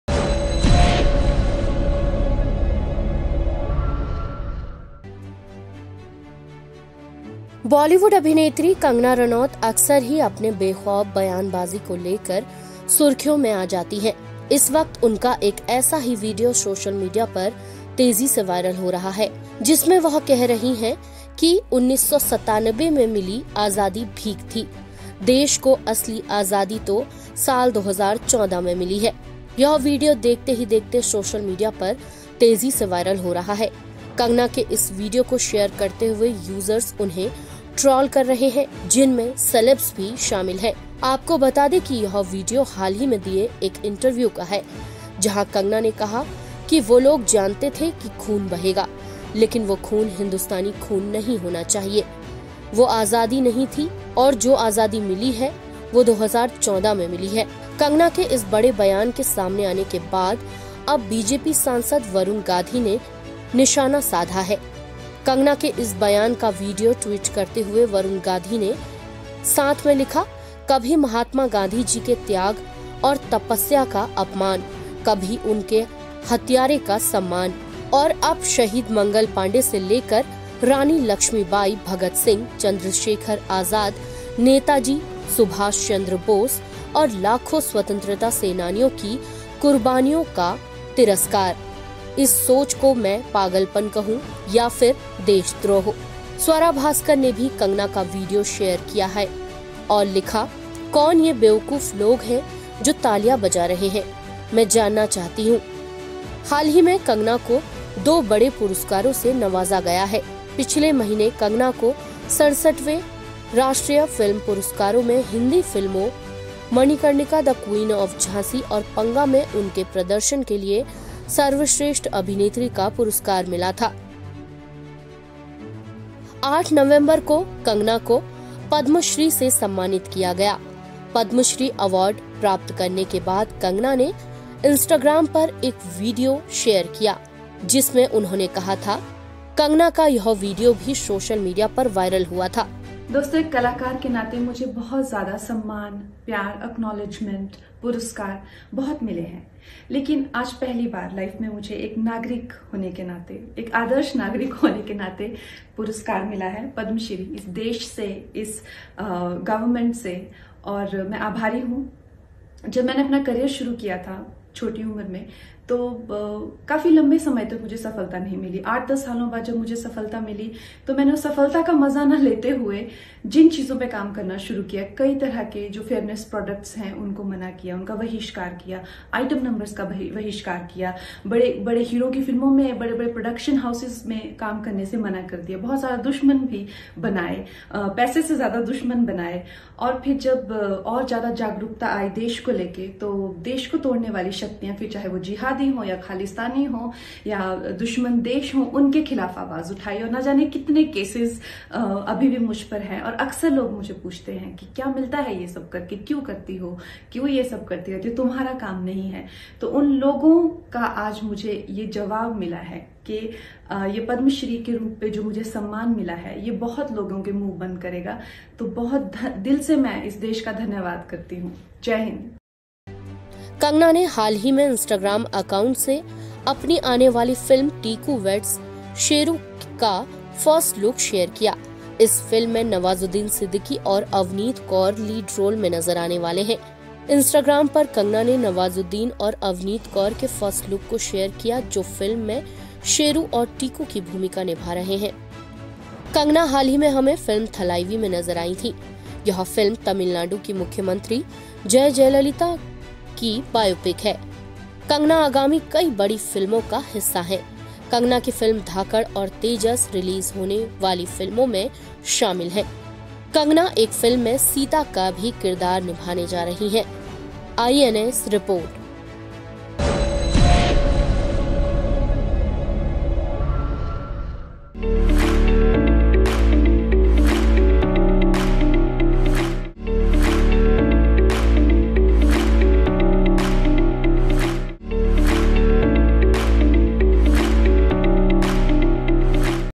Kangana Ranaut trolled over her another controversial remark
बॉलीवुड अभिनेत्री कंगना रनौत अक्सर ही अपने बेखौफ  बयानबाजी को लेकर सुर्खियों में आ जाती हैं। इस वक्त उनका एक ऐसा ही वीडियो सोशल मीडिया पर तेजी से वायरल हो रहा है , जिसमें वह कह रही हैं कि 1947 में मिली आजादी भीख थी, देश को असली आजादी तो साल 2014 में मिली।